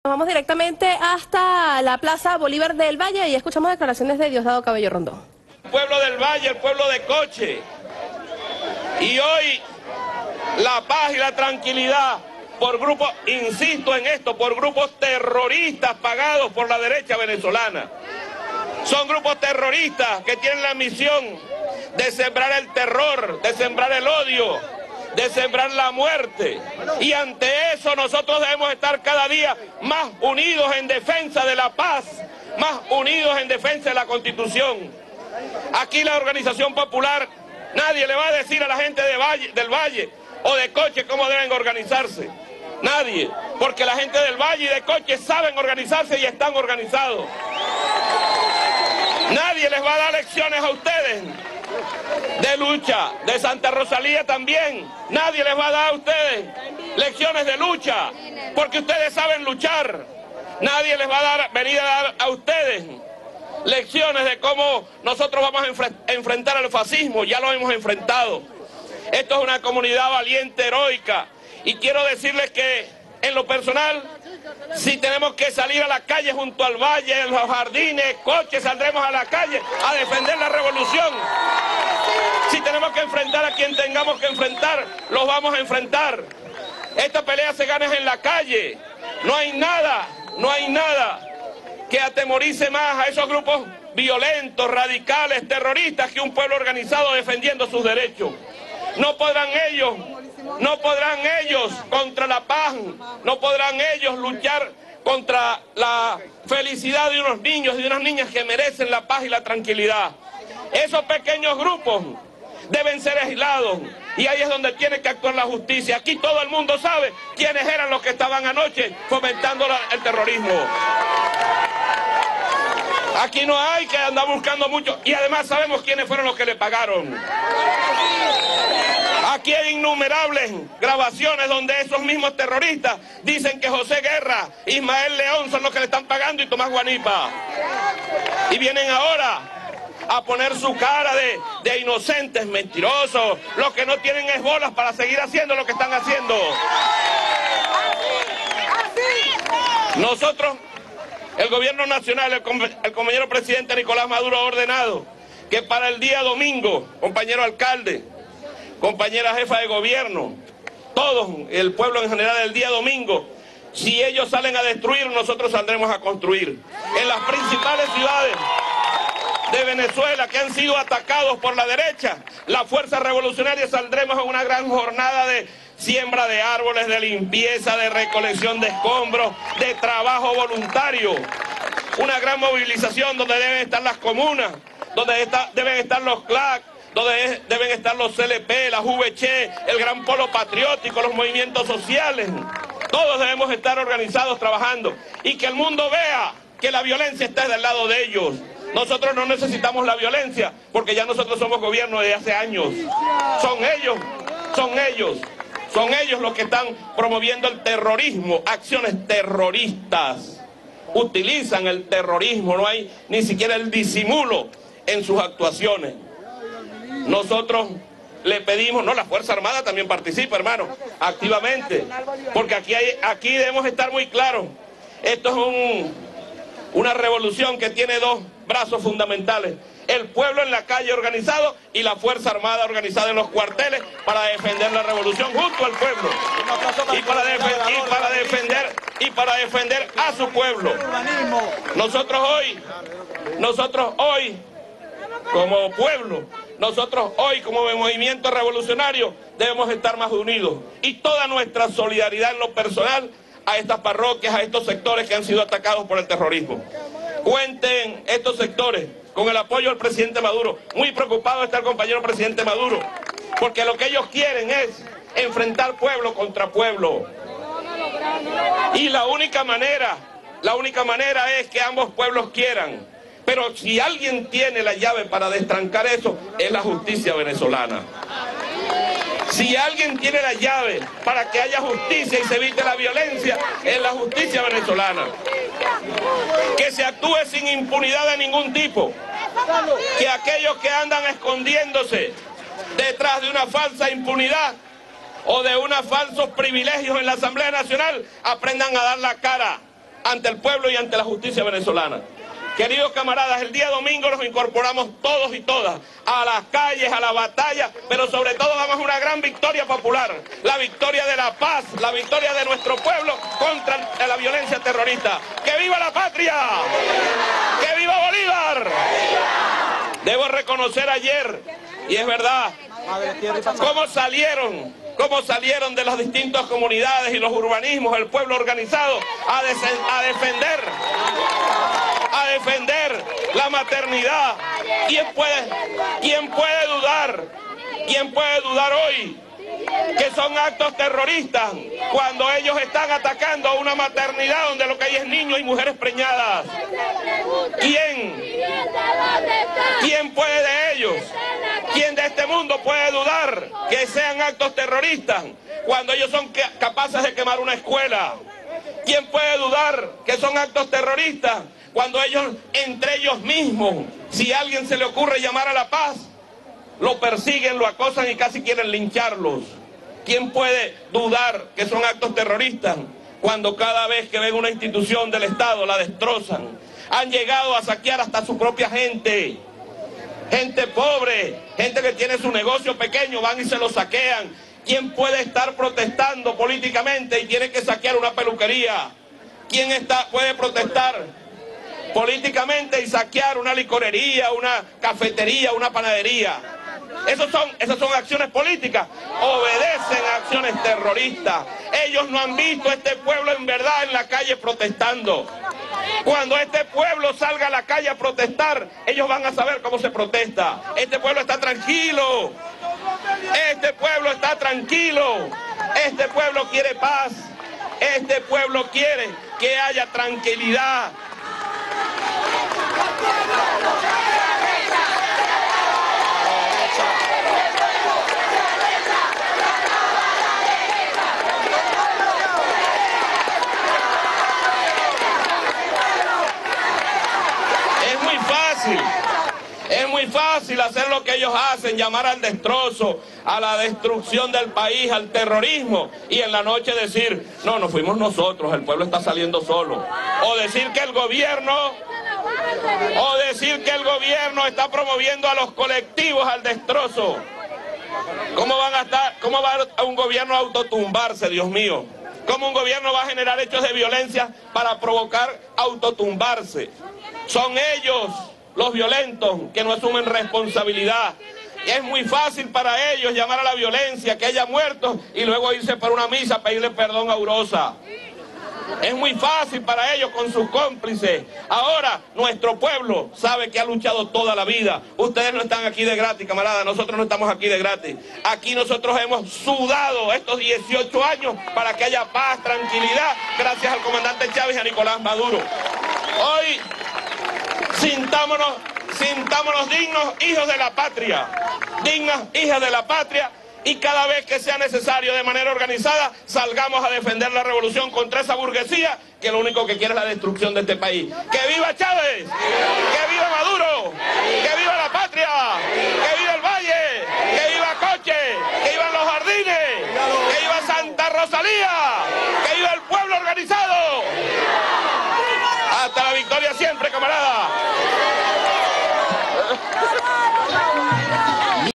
Diosdado Cabello desde la plaza Bolívar de El Valle tras hechos violentos en Materno Infantil
Palabras del diputado y vicepresidente del PSUV Disodado Cabello el 21 de abril de 2017, tras los hechos violentos del día anterior, que incluyeron saqueos y un ataque a un hospital Materno Infantil.